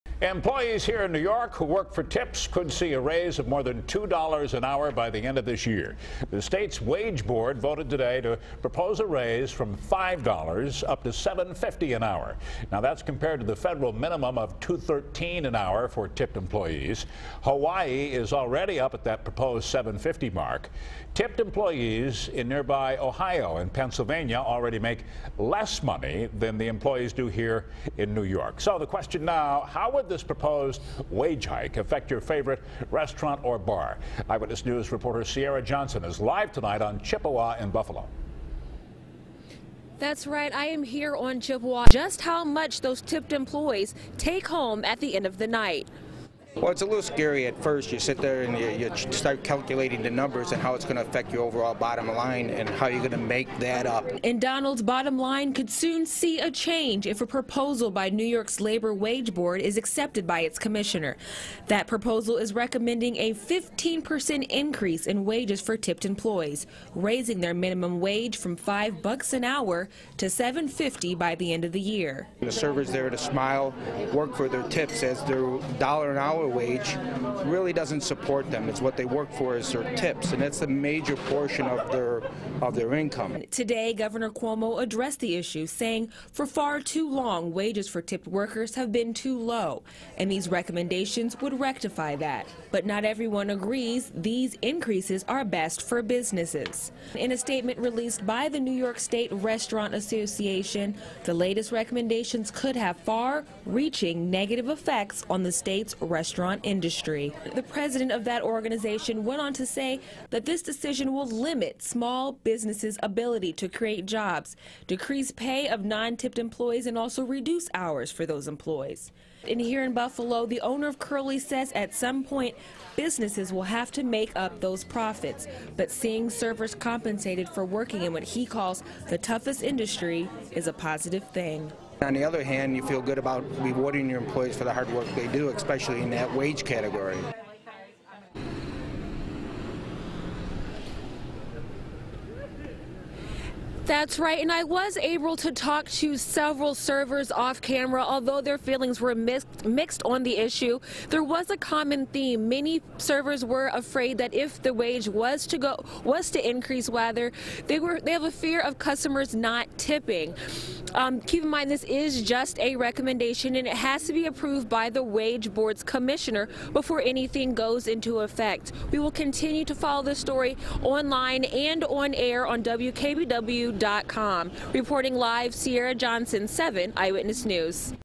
Wage board suggests $7.50 wage for tipped employees
7 Eyewitness News is Buffalo's Source for news, sports, weather, and good things happening every day.